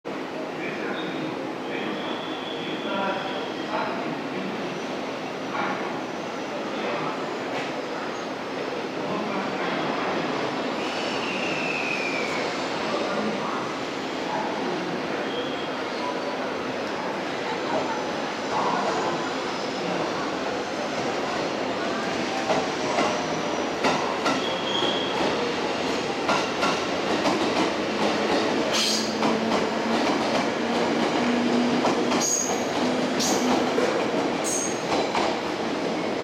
JR西日本 京都支所221系K12更新編成 ＠京都駅 2013/01/31
京都駅を出発する吹田総合車両所京都支所の221系更新第一編成です。この編成は京都寄クモハ221-73が冬季用パンタグラフを装備したダブルパンタ仕様になっているのが特徴です。